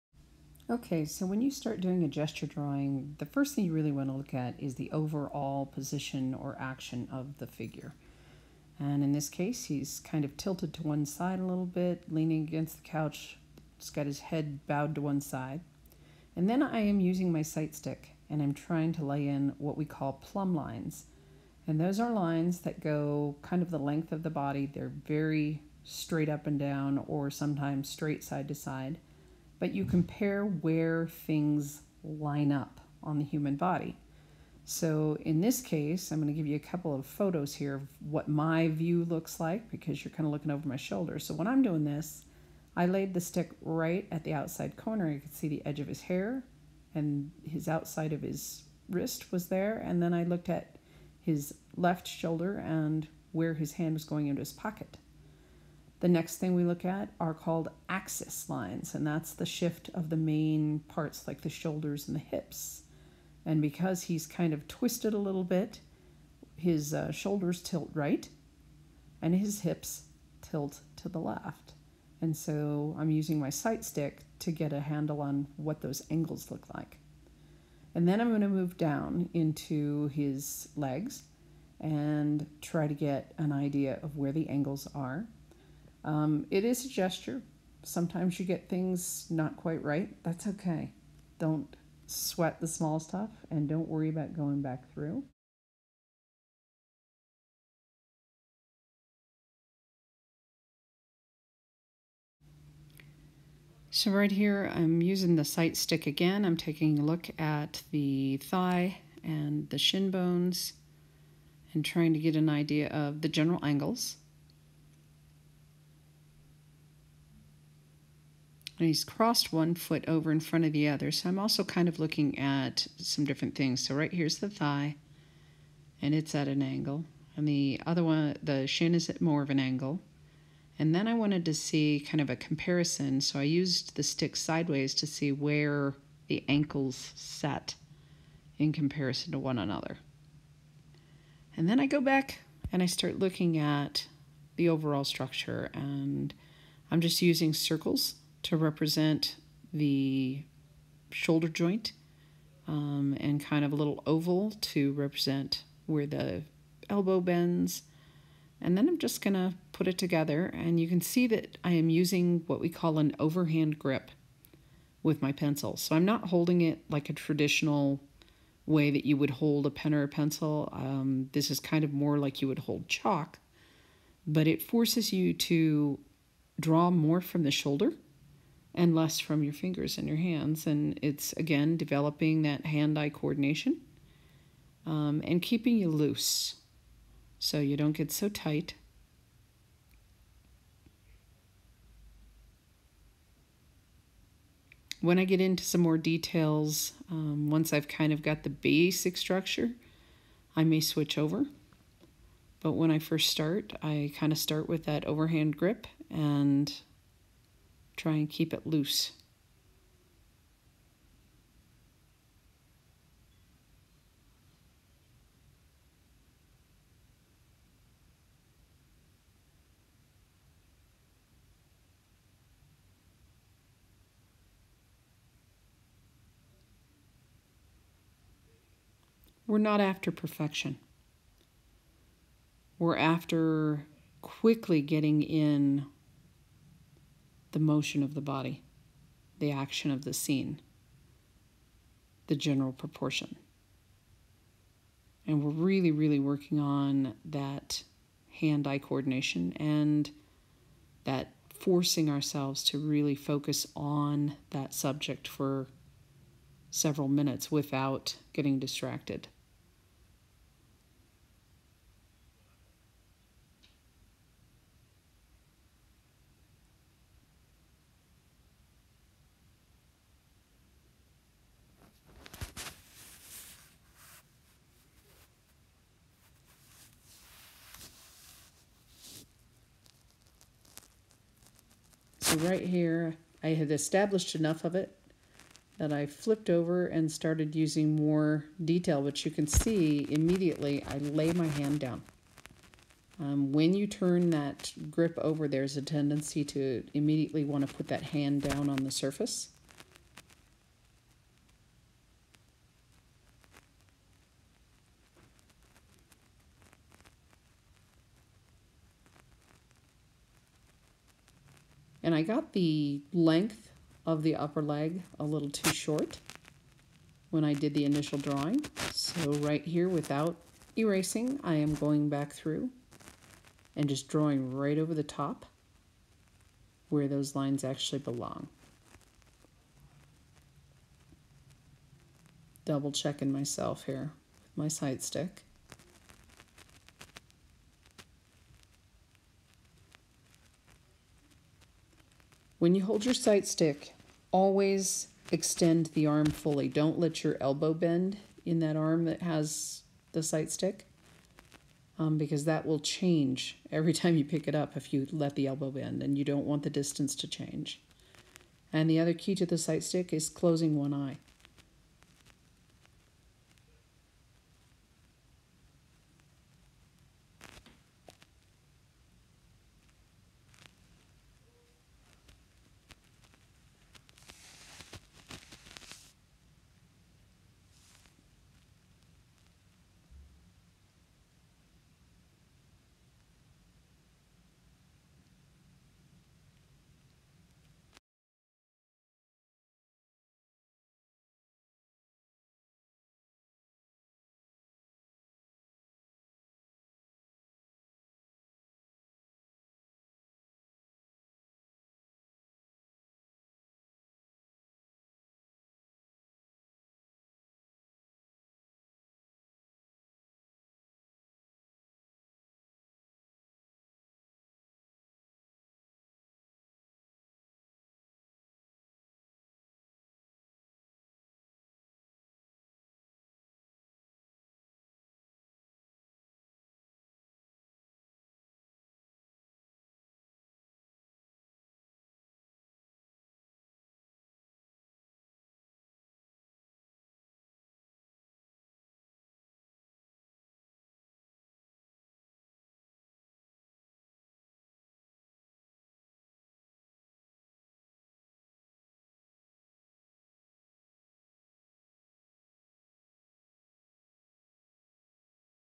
Using a sight stick and Gesture
Gesture drawing, using a sight stick, using plumb lines